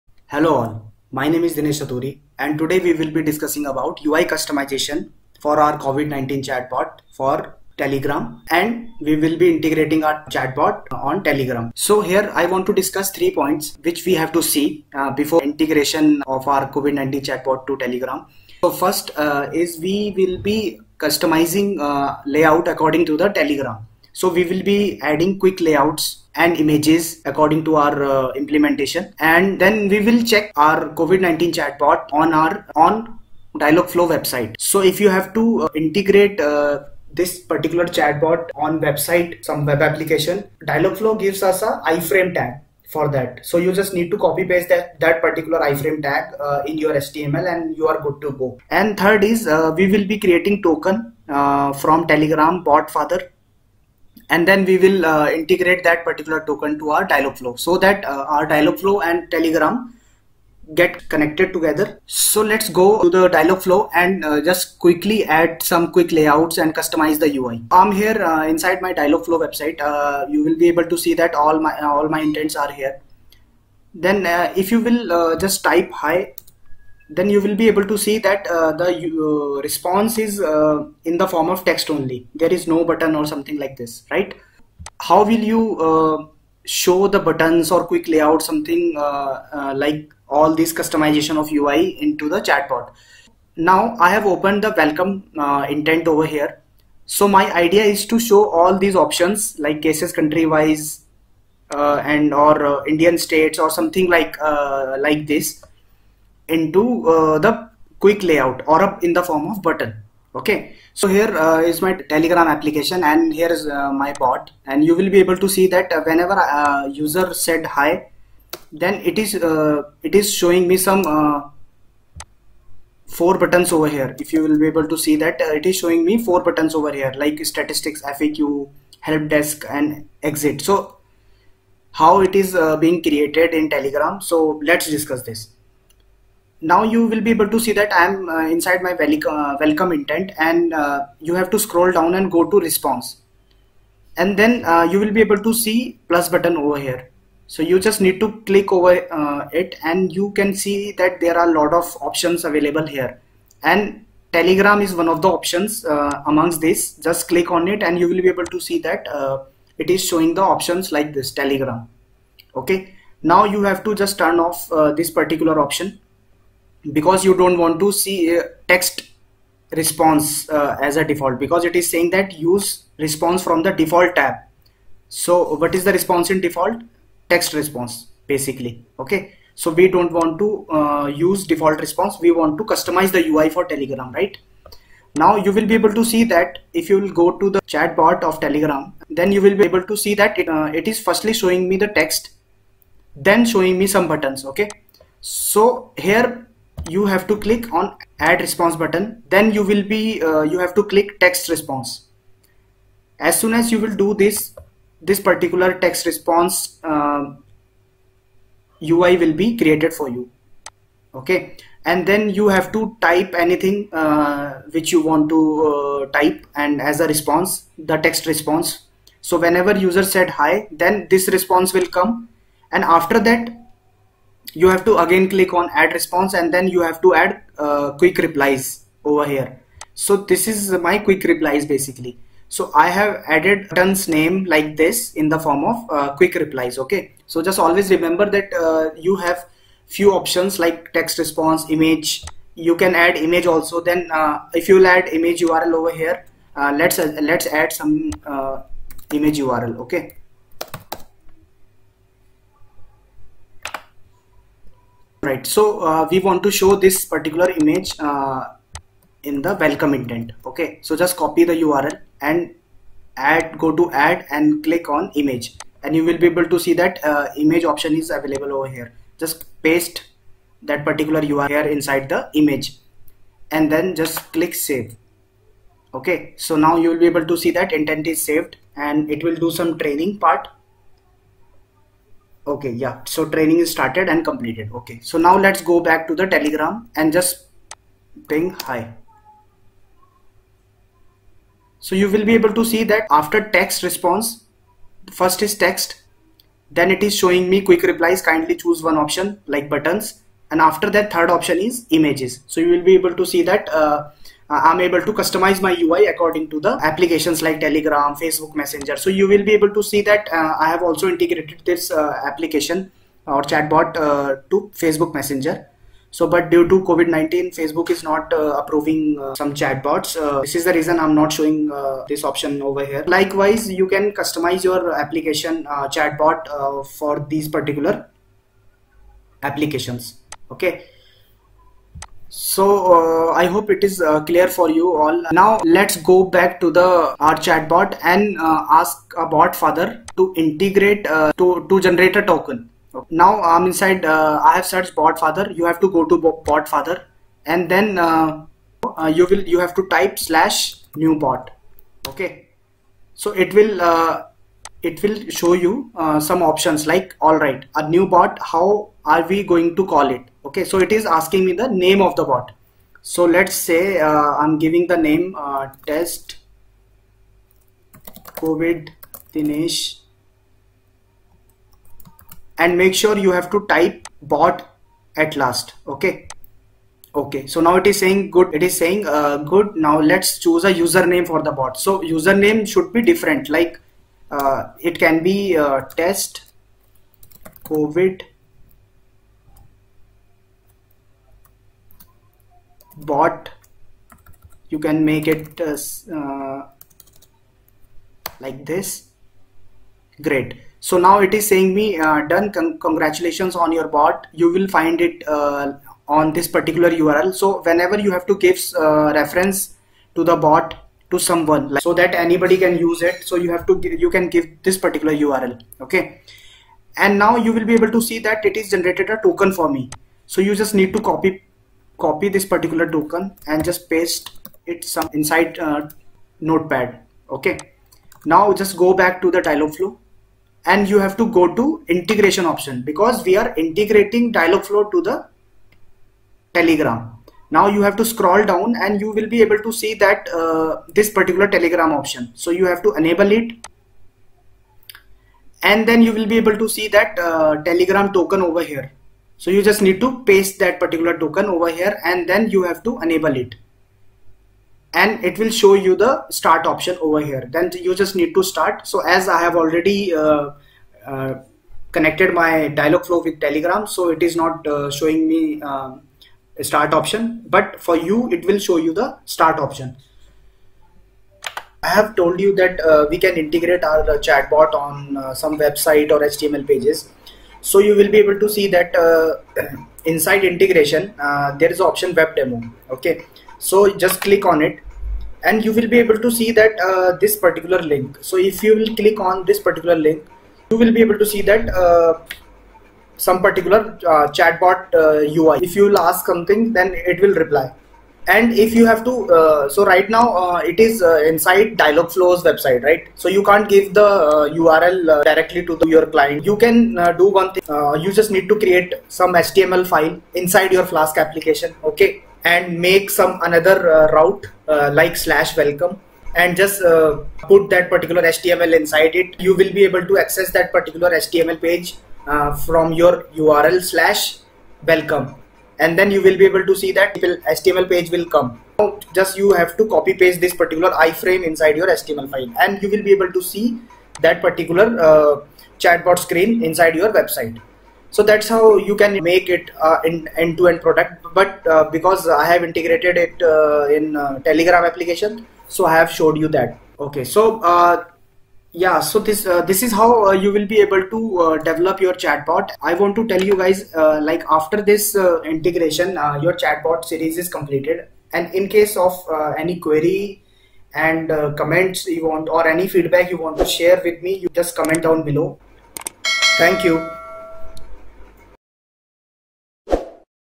Final Part |Chatbot Integration with Telegram,web|Quick Replies,Images to Response |Covid-19 Chatbot
This is the Part -6 of the Dialogflow Covid-19 chatbot series where I have Customized the UI for Telegram and Integrating It with Telegram.
I have also shown you how to integrate the chatbot with the website.

I have talked about Rich messages for FB messenger and telegram for that you have to use inline editor and customize your response from flask to Dialogflow.

2.
3. Create Free Rapid-Api account

Many people asked me regarding a Youtube series for the Covid-19 chatbot implementation over Linkedin

Our Task -
1. The bot should be able to answer all the queries/FAQ related to Covid-19

2. The bot should be able to show statistics worldwide, Country-wise, Indian State, District, On GoogleMap using 3rd Party API.

3. Able to send Report to User Email with Prevention Measure Attachments.

4.Able to show Help Desk, Live News, Govt Announcements, Images and Videos related to Covid-19.

5. Able to Save User-Bot conversation on Database.
Kindly hit the start button on my GitHub link if you like my work.